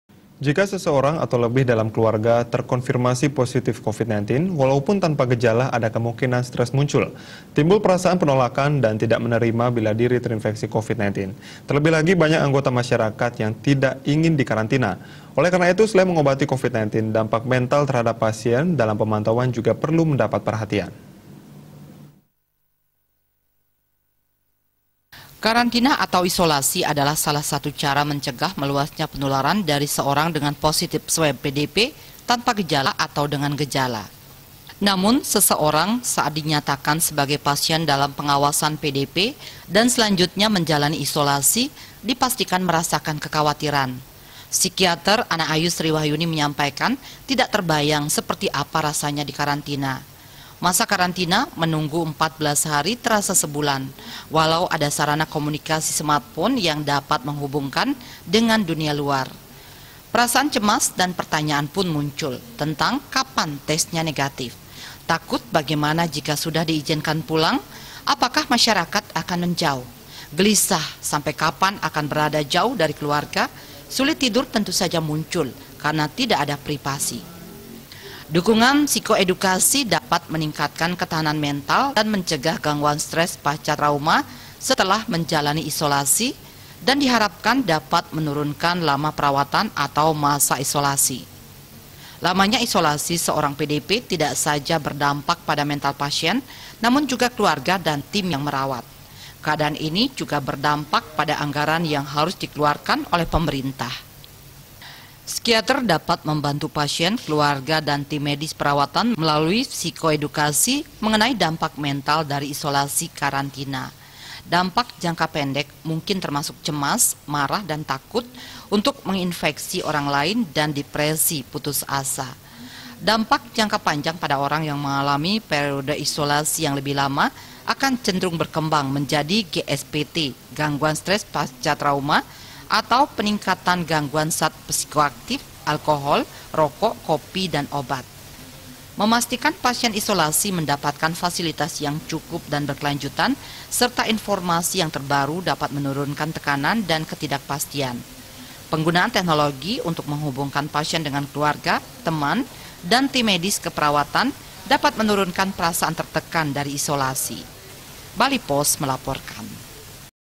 DAMPAK MENTAL ISOLASI PASIEN DALAM PEMANTAUAN
JIKA SESEORANG ATAU LEBIH DALAM KELUARGA TERKONFIRMASI POSITIF COVID-19, WALAUPUN TANPA GEJALA, ADA KEMUNGKINAN STRESS MUNCUL. TIMBUL PERASAAN PENOLAKAN DAN TIDAK MENERIMA, BILA DIRI TERINFEKSI COVID-19. TERLEBIH LAGI, BANYAK ANGGOTA MASYARAKAT TIDAK INGIN DIKARANTINA. OLEH
KARENA ITU, SELAIN MENGOBATI COVID-19, DAMPAK MENTAL TERHADAP PASIEN DALAM PEMANTAUAN JUGA PERLU MENDAPAT PERHATIAN.